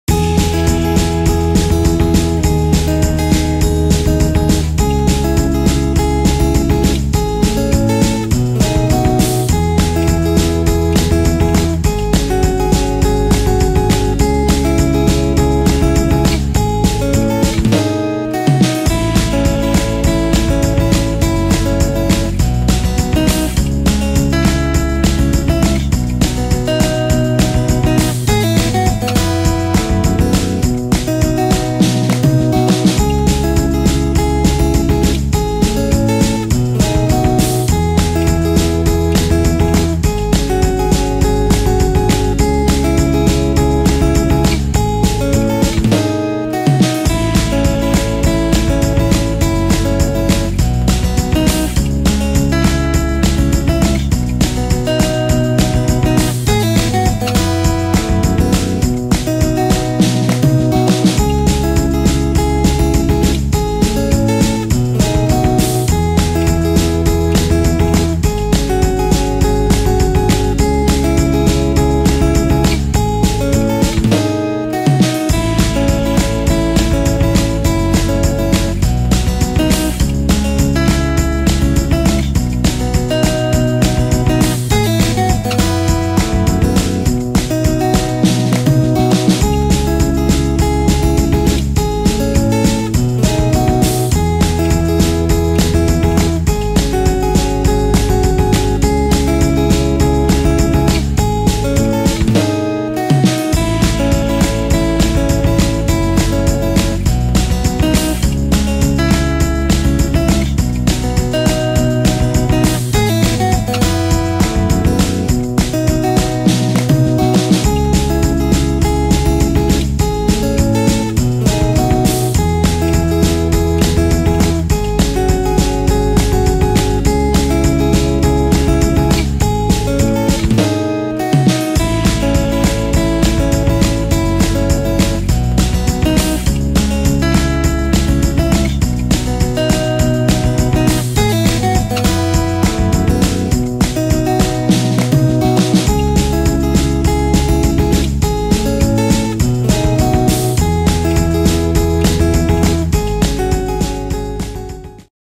A fun day in the snow for the suzuki samurai's to play!
The ruts in the snow drifts are getting deep in the spring.